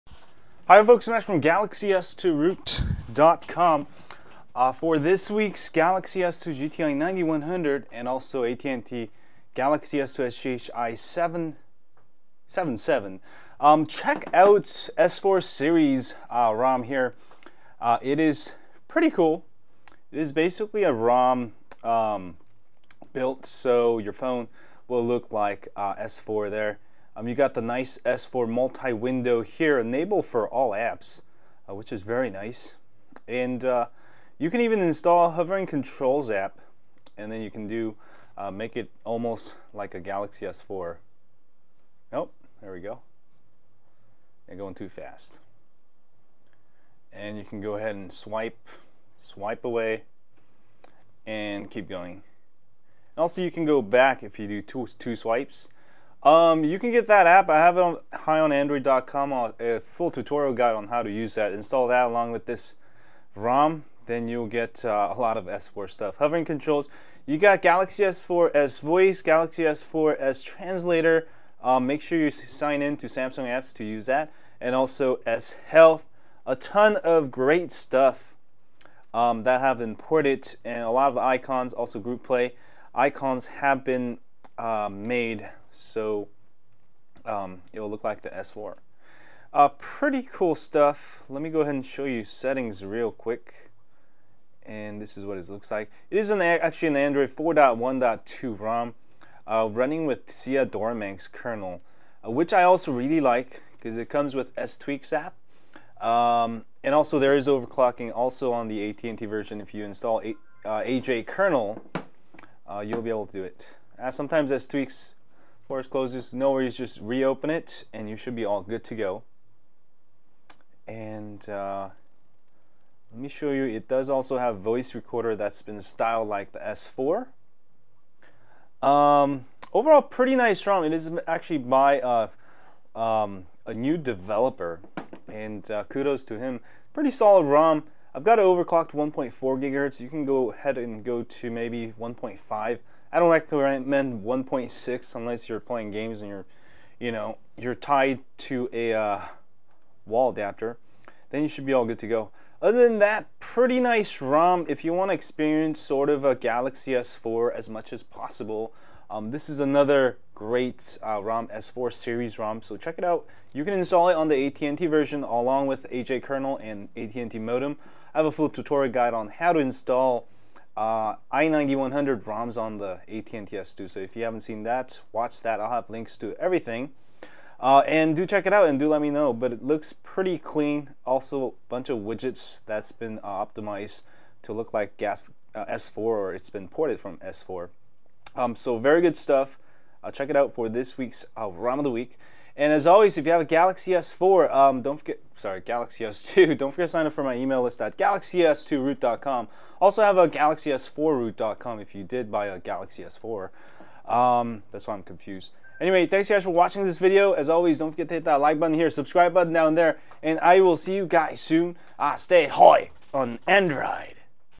SIV Series ROM for Galaxy S2!
Here's a quick overview of SIV Series ROM for Galaxy S2 Android smartphone users.